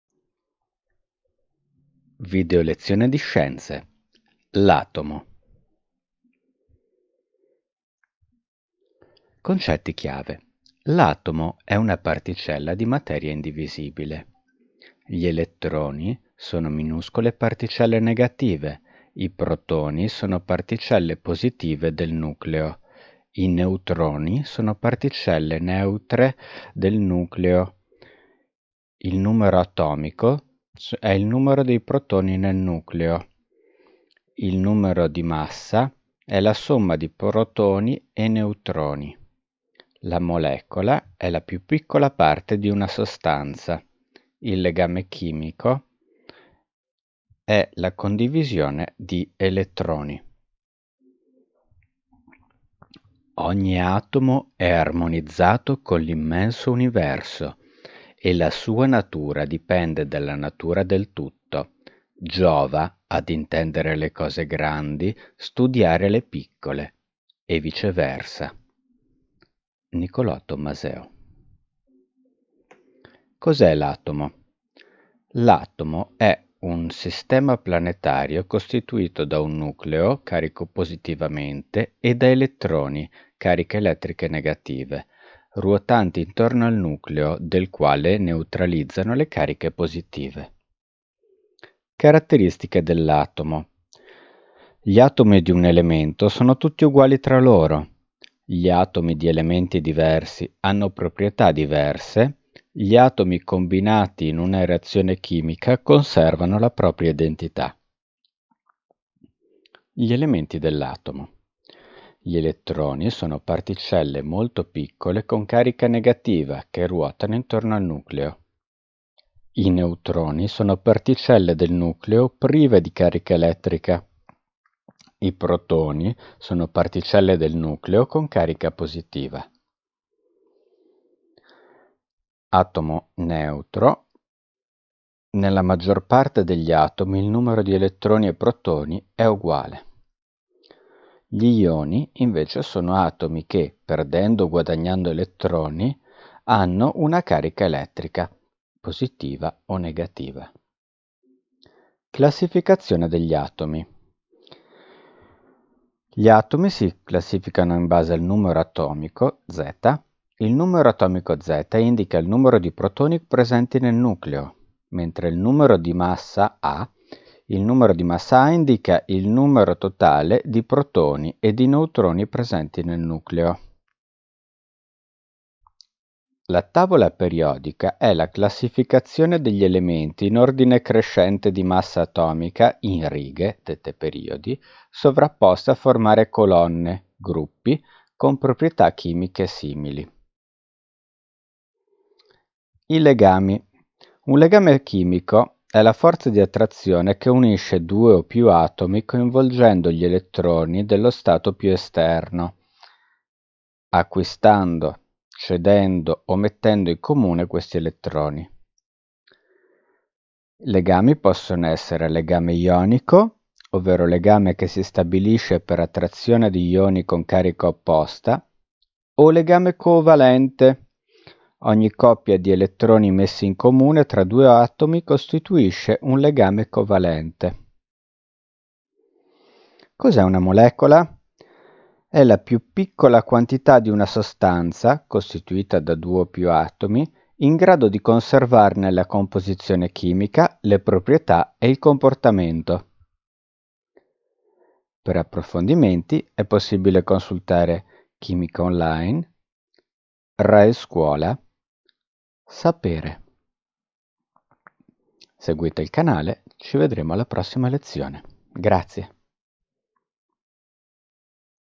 SCIENZE - l'atomo
Videolezione di ripasso e consolidamento sull'argomento di Scienze "l'atomo".